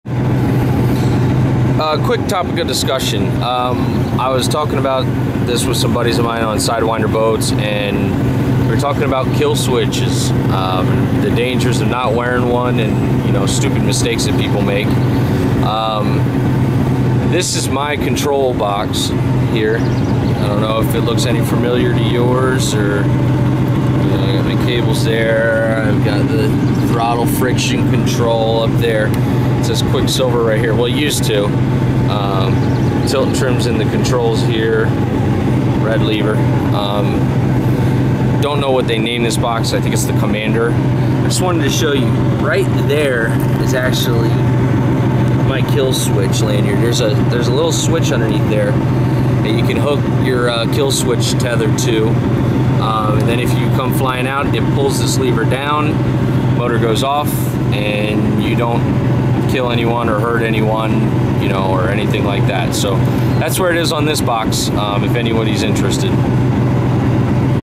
Location of Kill Switch Lanyard
Just showing some people where you are supposed to attach your kill switch lanyard.